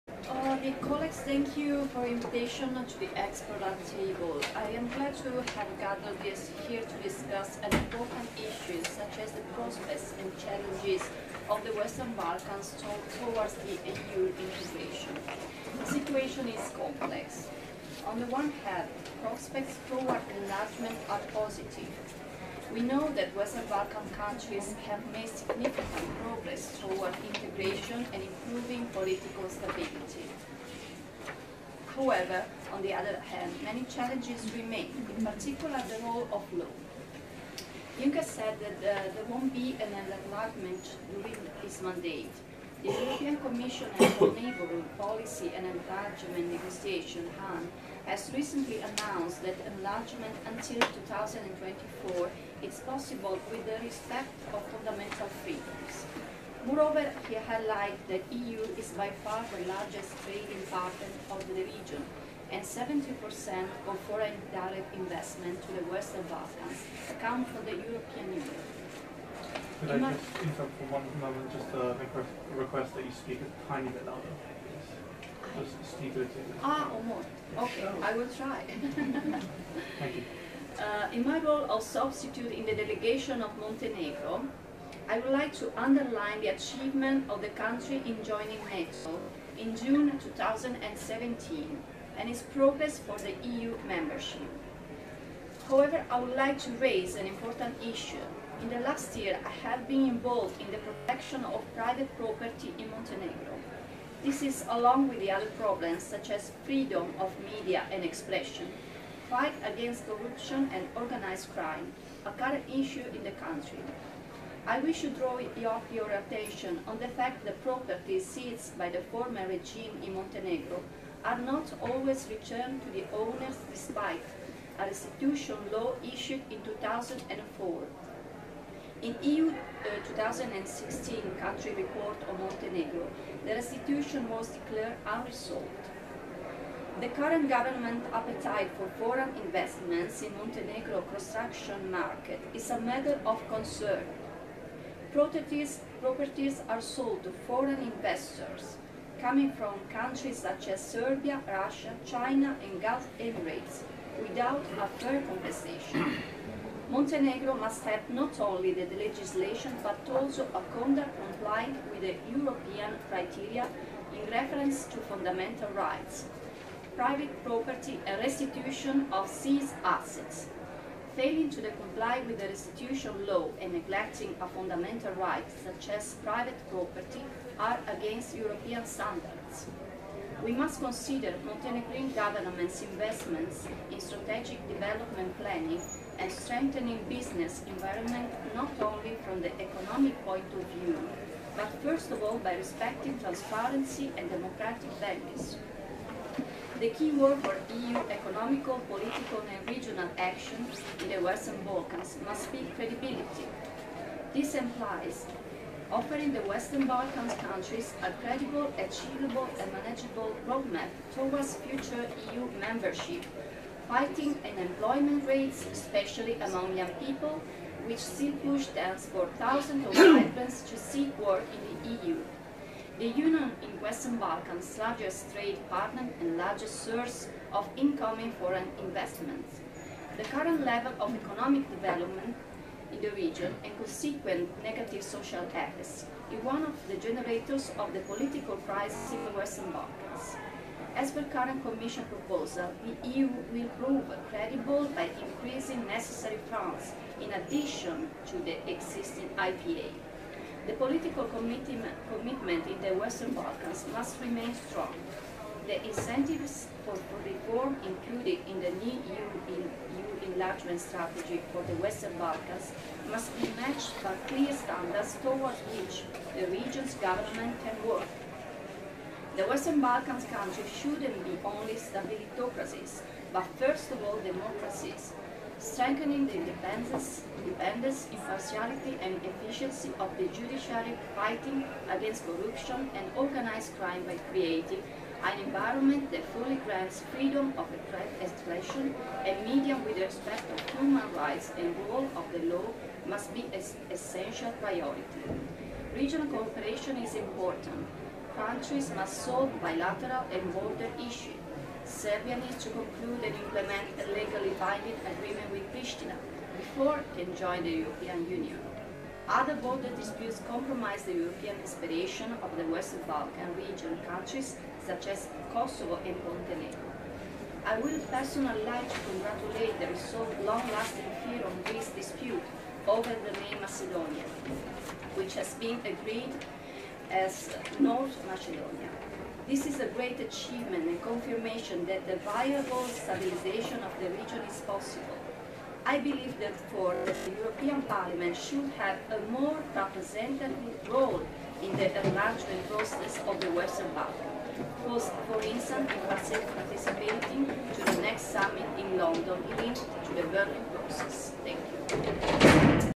Isabella DE MONTE, Substitute of European Parliament Delegation to Montenegro
Isabella DE MONTE shares hER take on the recent state of the relations between Montenegro and the EU at the panel organized by Vocal Europe.